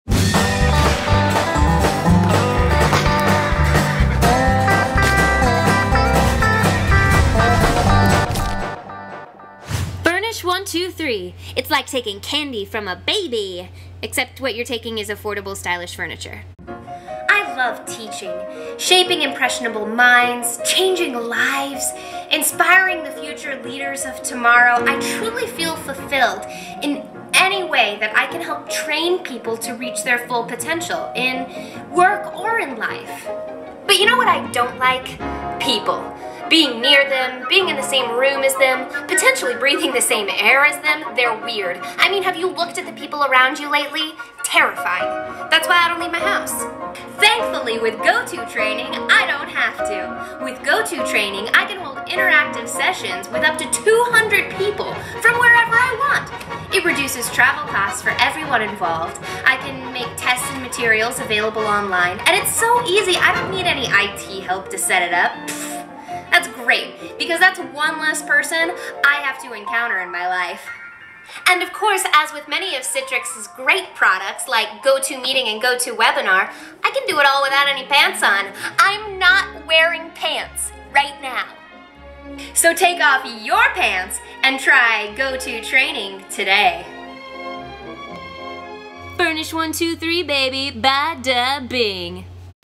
GoToTraining and Stay Away from People!
GoToTraining can help you save time and travel costs by moving your training programs online. Whether it's HR training, IT training, new employee training, etc, you can train people anywhere around the world. Need to demonstrate how to use a new company software or tool? GoToTraining is interactive, easy to follow along and you can track the trainee's comprehension and performance. Want to reach new customers without having to have a brick and motar location? GoToTraining works with Mac and PC, nearly any web browser available and they offer 24/7 customer support if you need it.

You can promote your online training sessions with GoToTraining as free or paid. If you choose paid you can setup flexible payment options, automated reminder emails and give access to all training materials used (including session recordings).